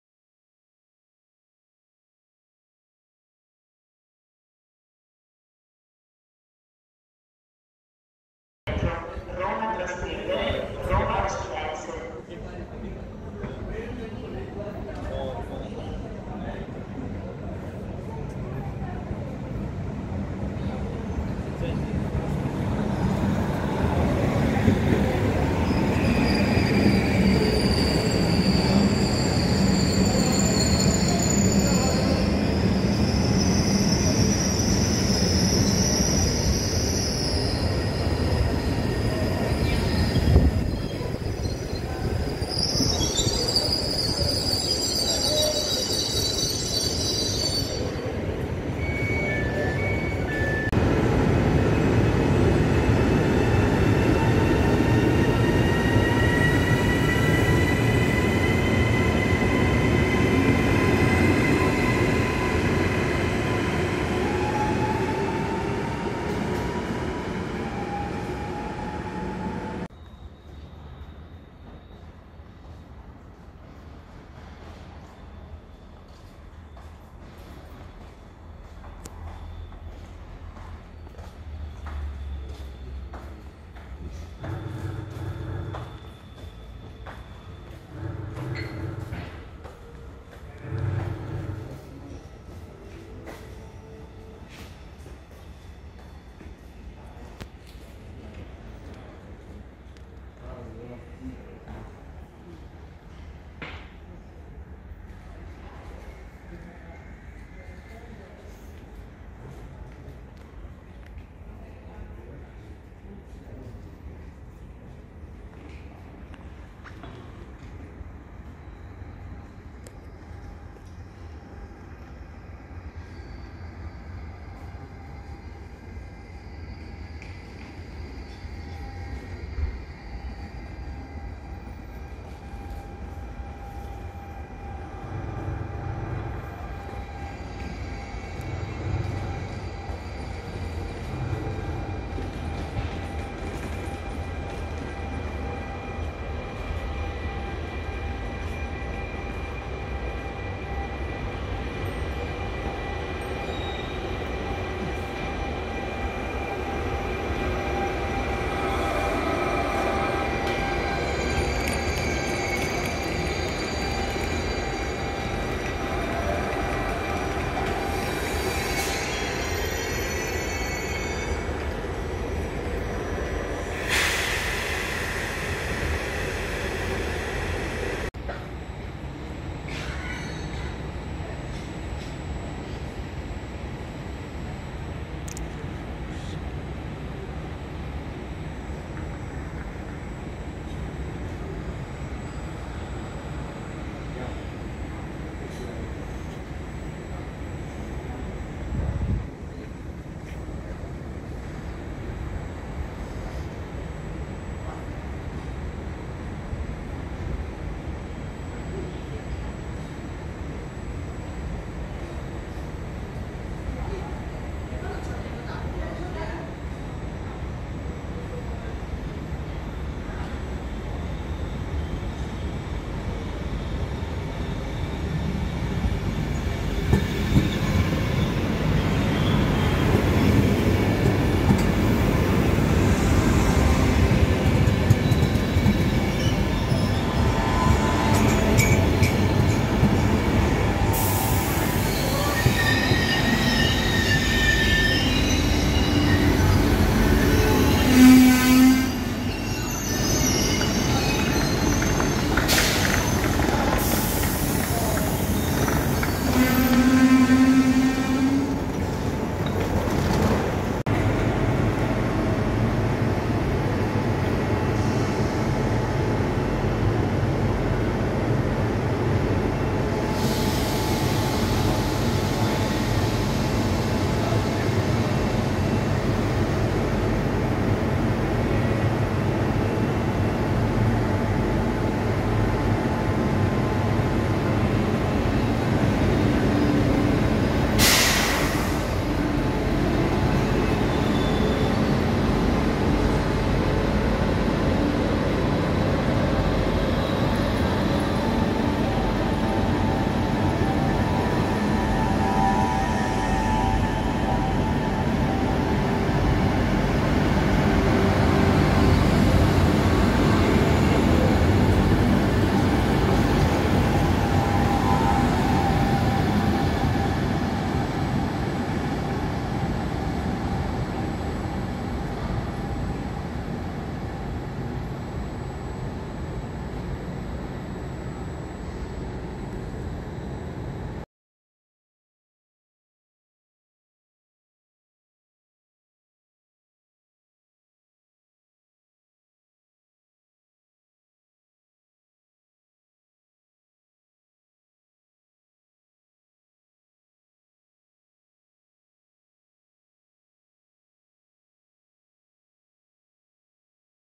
Grosseto - Arrivi e partenze il 1° Settembre 2024
Breve compilation di filmati realizzati la mattina del 1 Settembre 2024 con Samsung A33 e montati con CapCut

Vediamo il Regionale Veloce 4127 effettuato con Rock ETR 521, a seguire il materiale utilizzato per il successivo Regionale "Tuscany Line" 18298 in arrivo al binario 5, subito dopo il Frecciabianca 9805 in ritardo di 5 minuti ed effettuato oramai da qualche tempo da materiale Frecciargento ETR 485 mentre per gli ETR 460...chissà che fine faranno.

Nel finale, un paio di foto al binario 5 della E464.573 titolare del Tuscany Line di quel giorno.